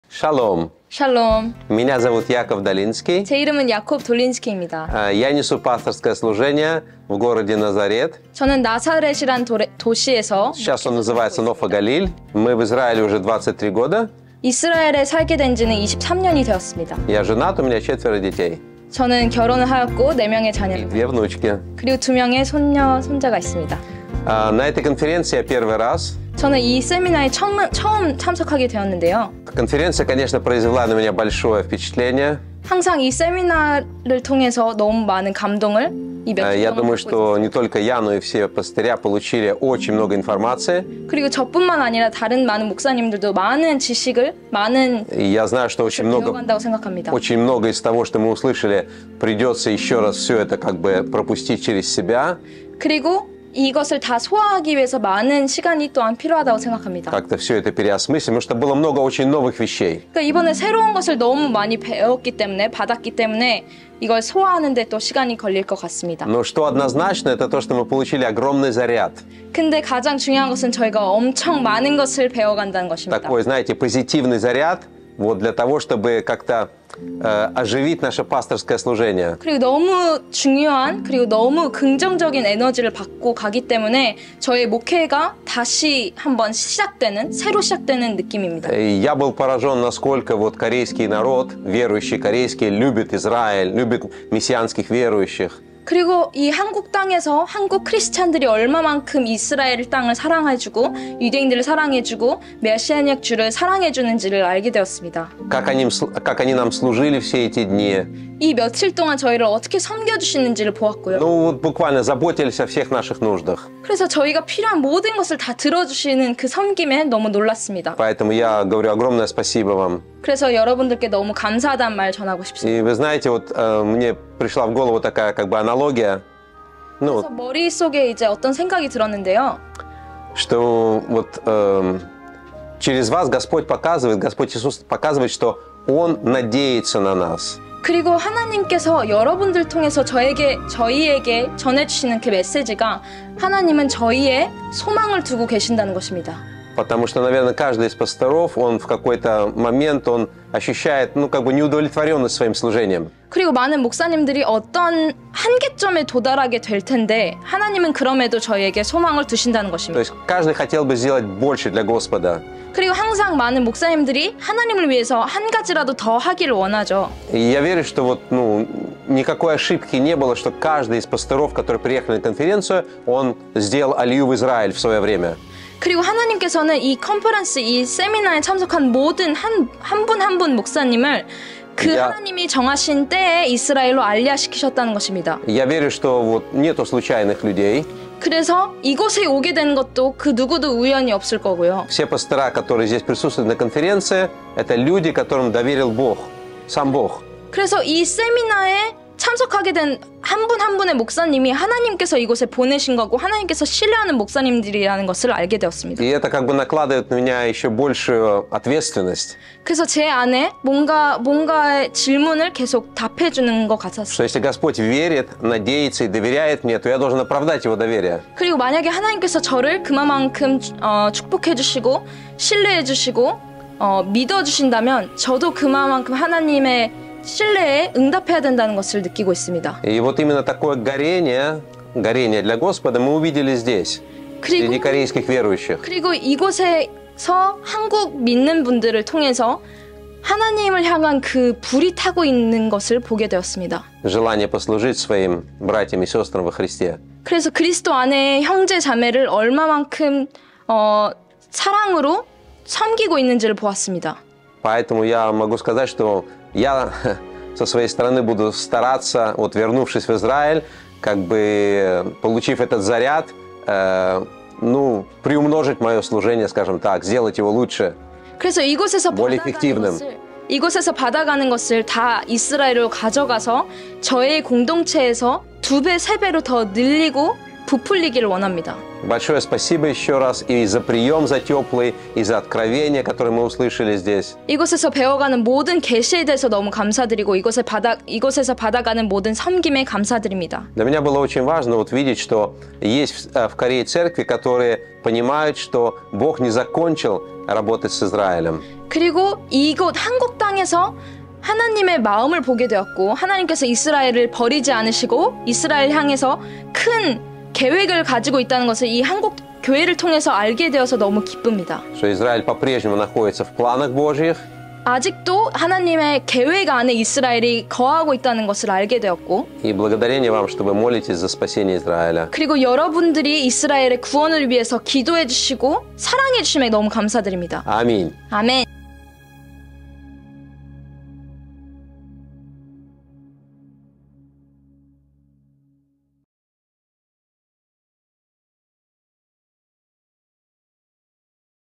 [인터뷰] 야콥 목사님의 세미나 간증⎮제 1회 이스라엘 목회자 컨퍼런스 🇮🇱⎮월드브릿지미션
📍 제 1회 이스라엘 목회자 컨퍼런스 영상 다시보기